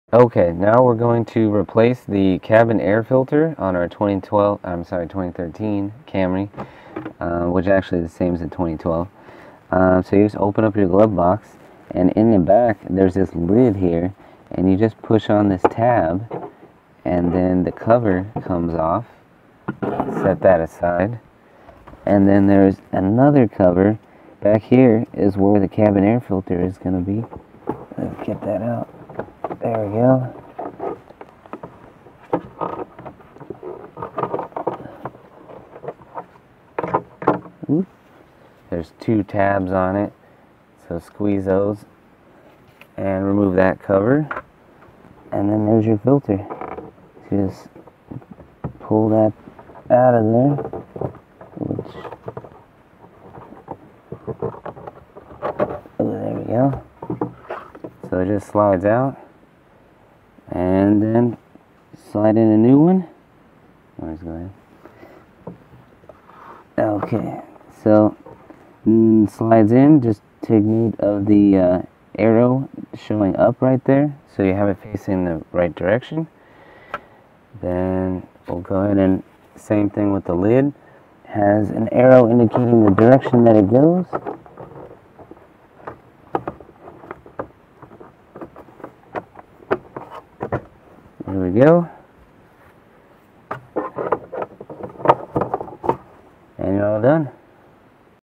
2012-2016 Toyota Camry cabin air filter change
2013 Toyota Camry cabin air filter replacement.

Tools:
Hands

Parts:
Cabin air filter

Torque specs:
None